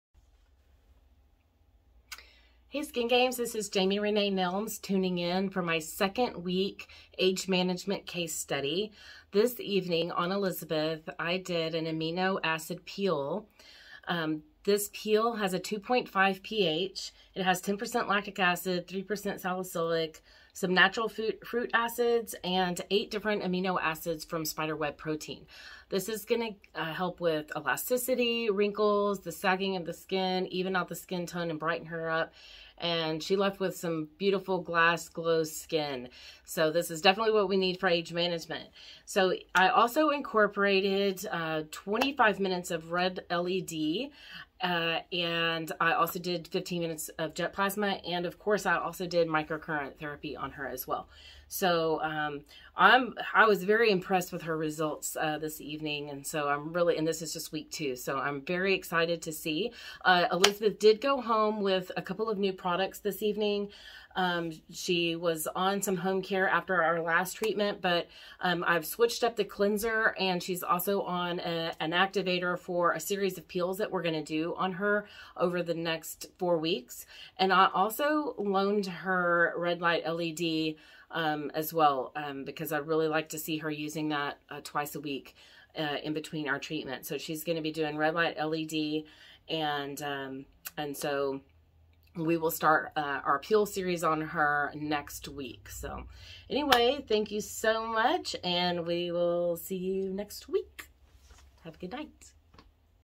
Jamie Renee Nelms treating Aging Skin in TSG2022 - video 2
Jamie Nelms updates us in the second week of her Age Management case study.

She did an Amino acid peel for he model which has lactic, salicylic, fruit and amino acids to help improve skin elasticity and saggy skin.

Additionally, Elizabeth, Jamie's model got LED therapy and Microcurrent treatment to complete the second visit.